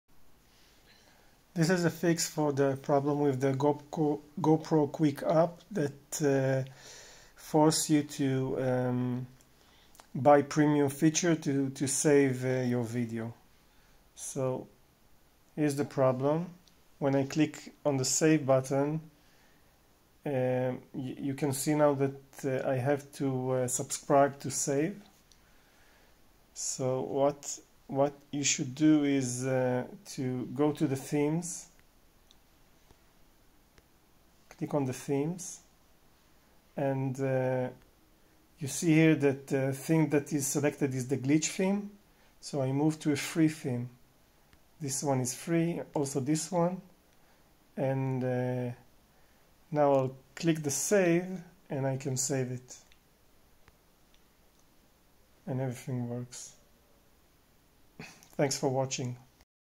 Fix for GoPro Quik app to avoid premium subscription
You must connect a gopro to the app in order to enable editing without trial or subscription to GoPro.